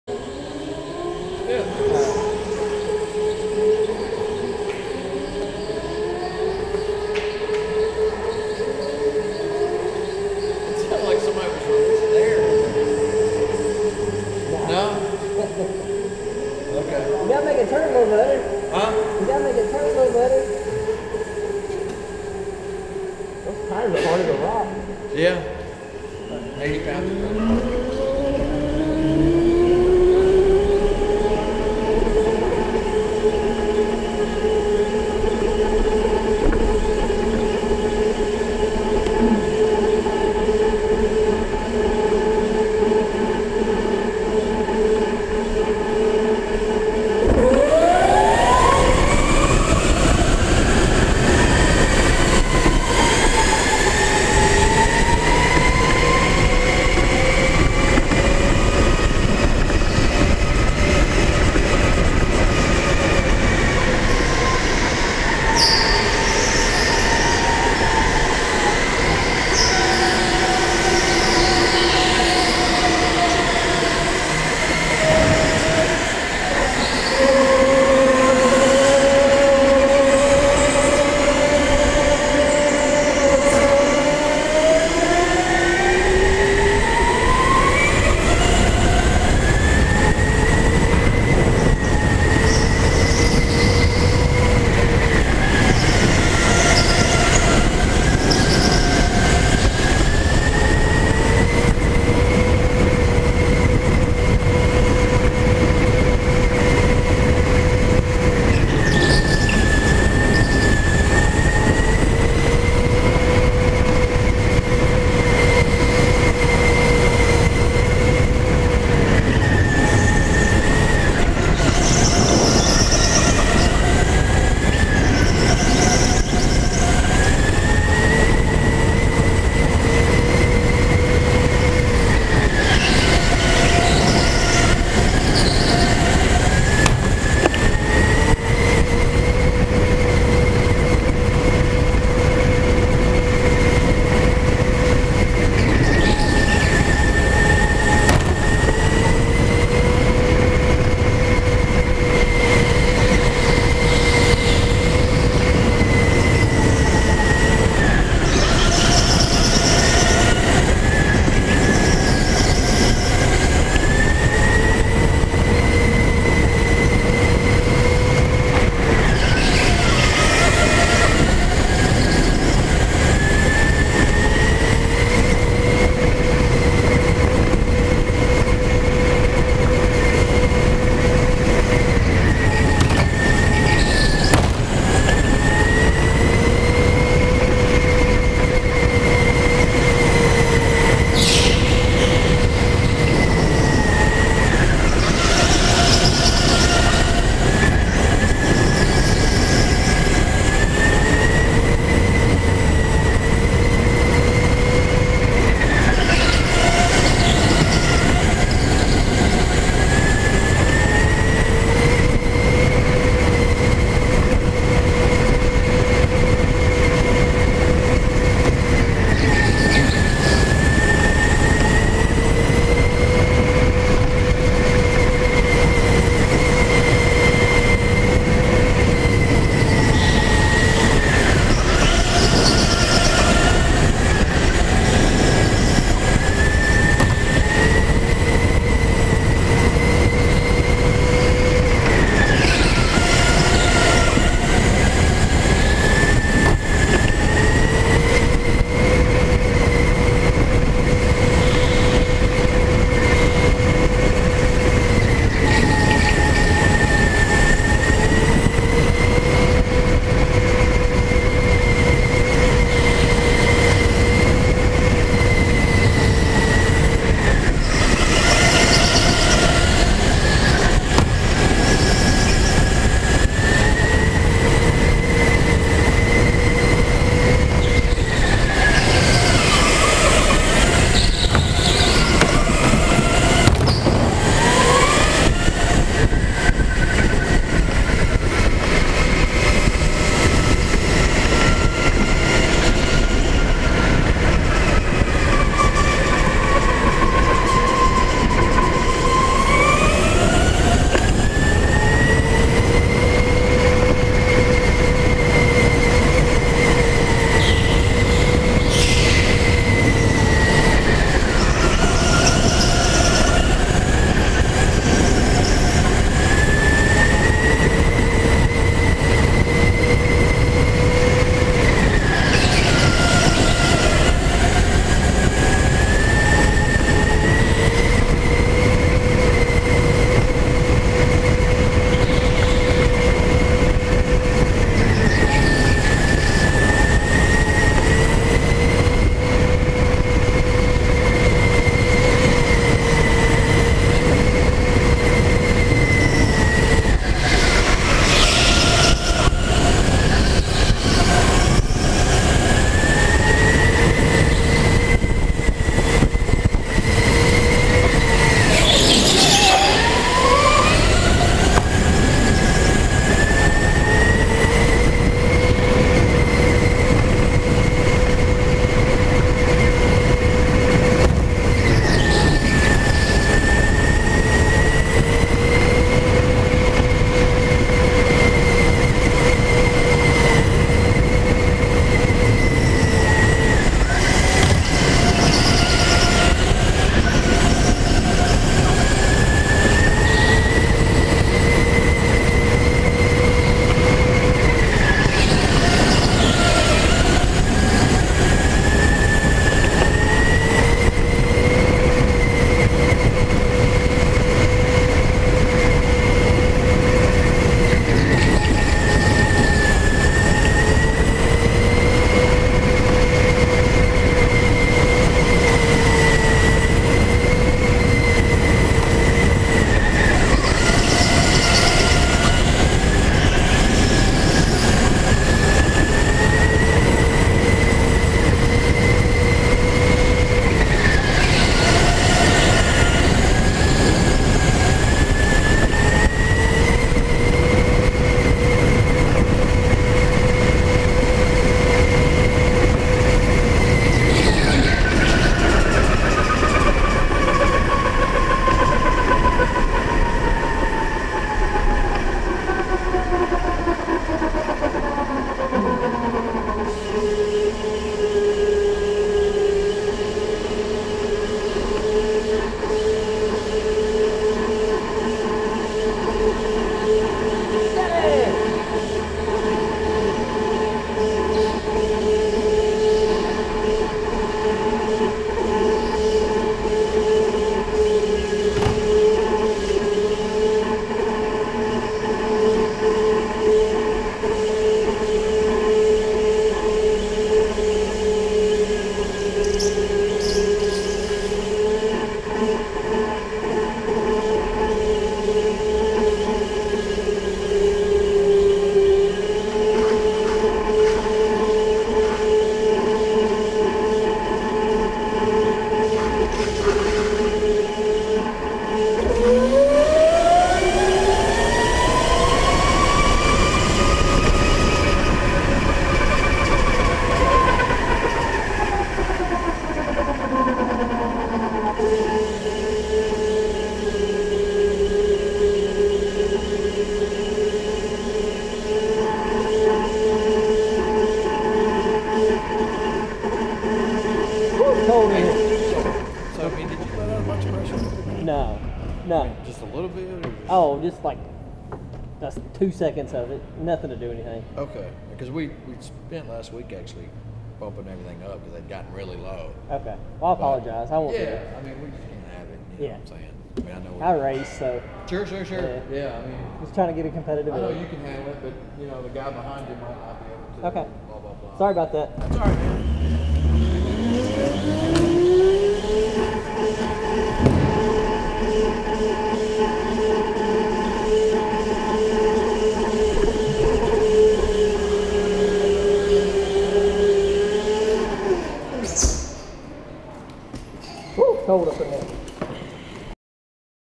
Stuart Kirby at Music City Indoor Karting
Stuart setting the fastest times of the day, 35.982.